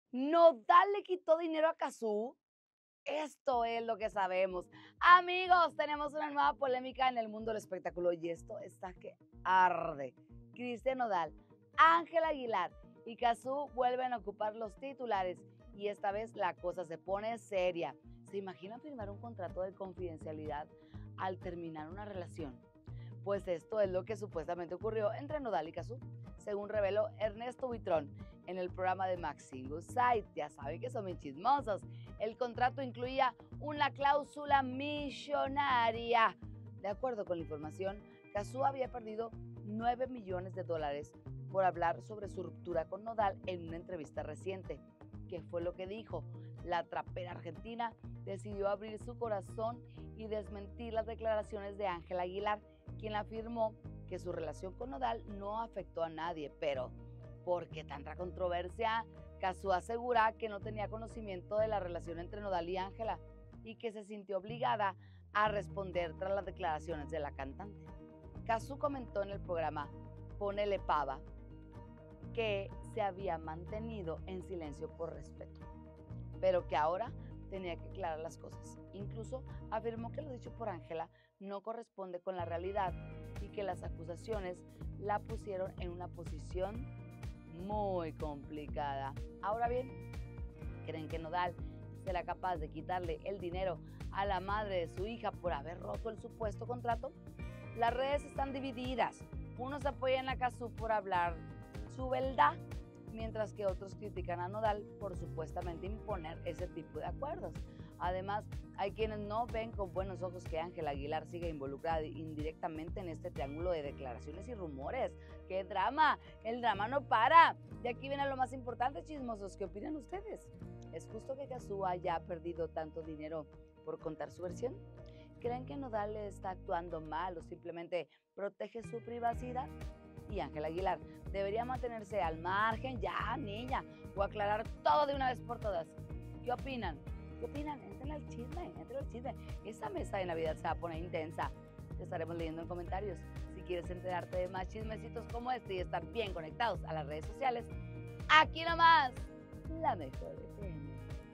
¿Nodal le quitó dinero a Cazzu?C: Esto es lo que sabemos…
Christian Nodal, Ángela Aguilar y Cazzu vuelven a ocupar los titulares, y esta vez, la cosa se pone seria. 😱

¿Se imaginan firmar un contrato de confidencialidad al terminar una relación?